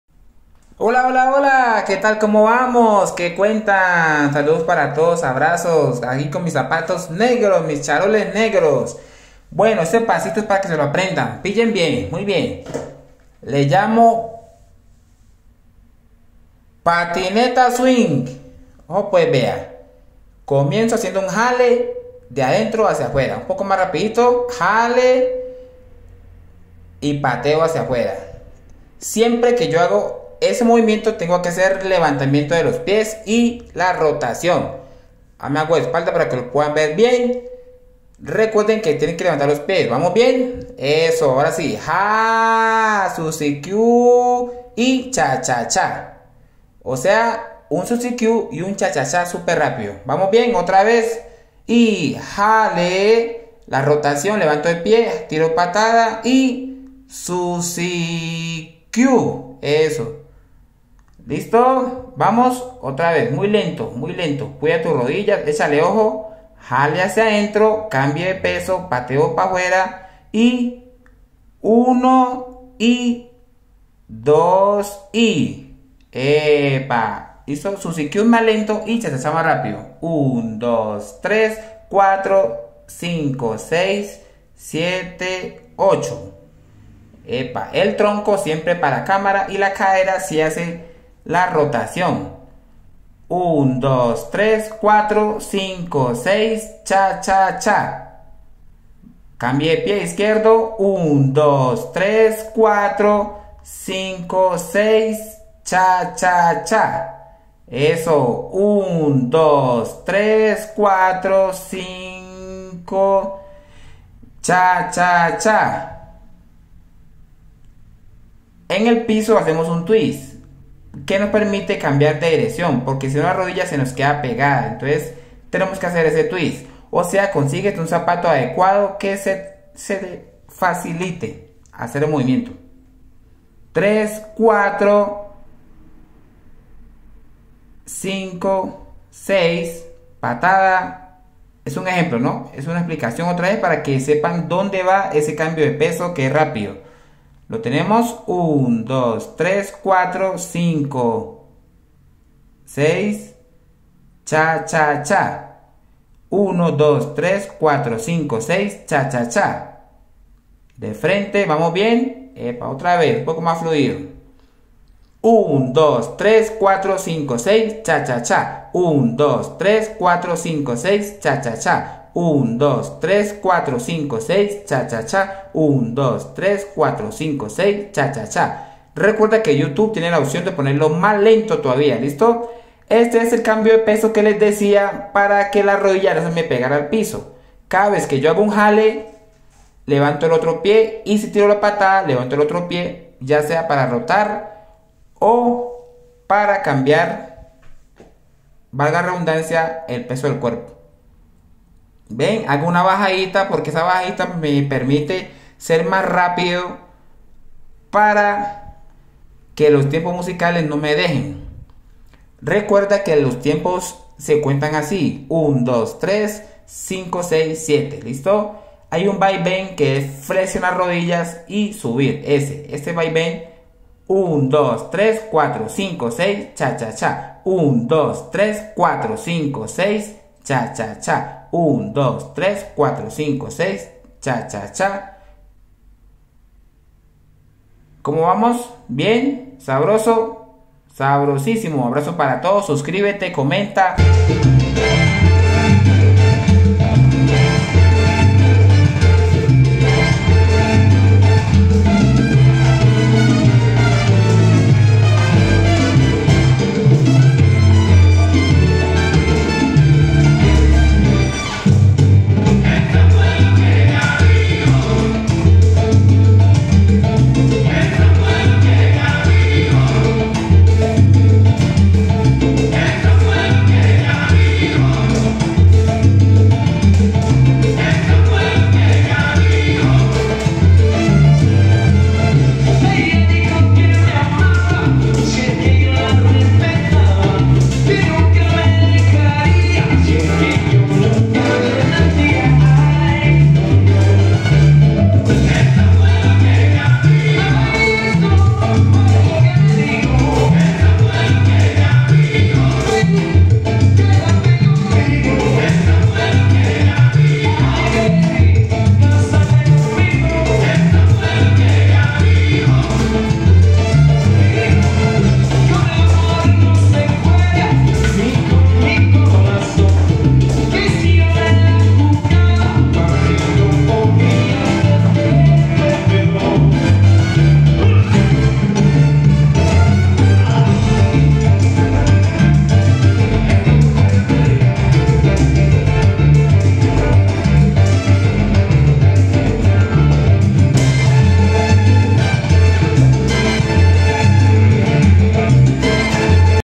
Aprende la PATISWING - Salsa caleña.
Aprende con nosotros a bailar salsa al modo de estilo caleño... Aprende sus vivcios, formas tips y pasos mas conocidos. Si necesitas clases de baile personalizada y luego sorprender a tus amigos, novio, novia , etc... Llamame y escribeme al correo: bailaconswing( arroba hotmail.com)